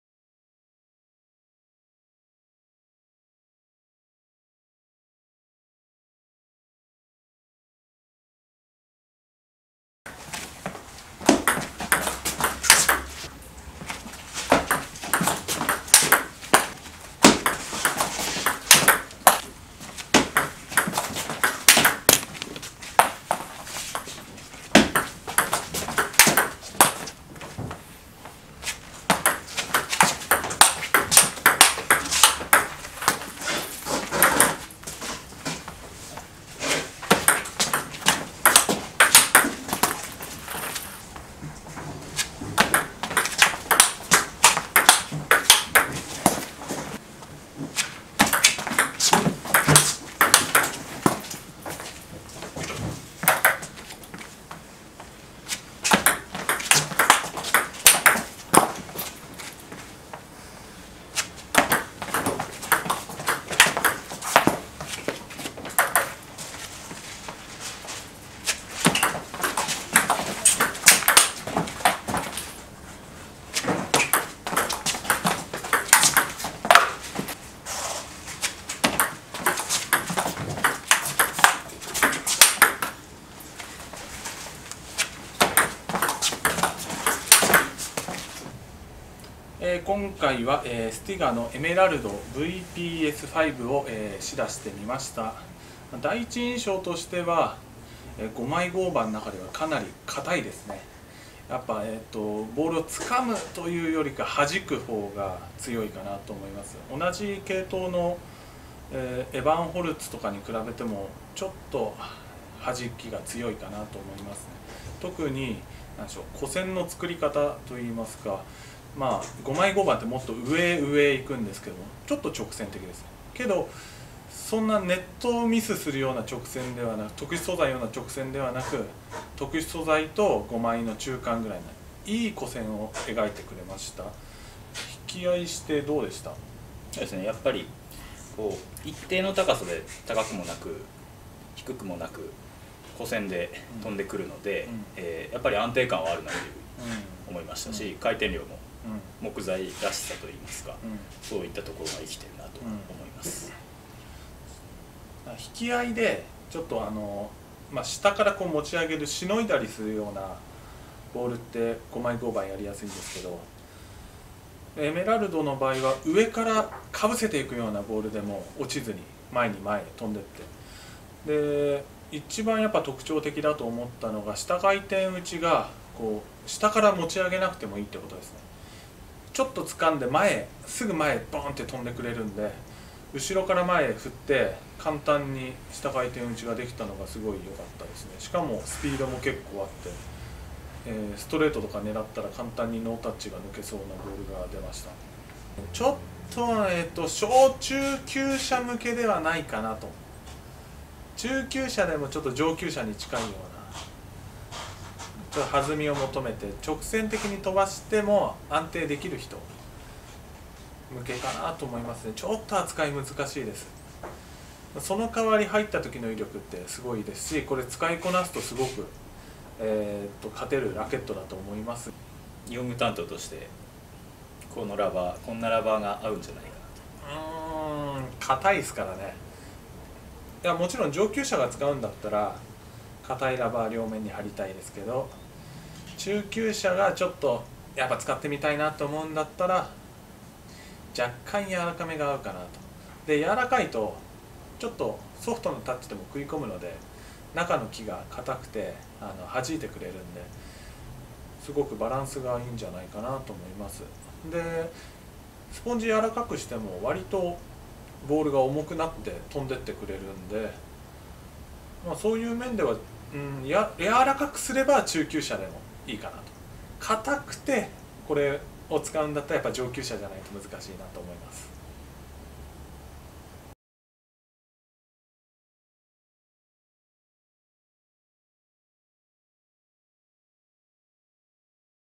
【卓球王国】エメラルドVPS V［スティガ］試打
卓球王国2015年2月号掲載『Editor's EYE』で紹介したエメラルドVPS V［スティガ］の試打リポートをお送りします！

卓球王国2015年2月号の詳細はこちら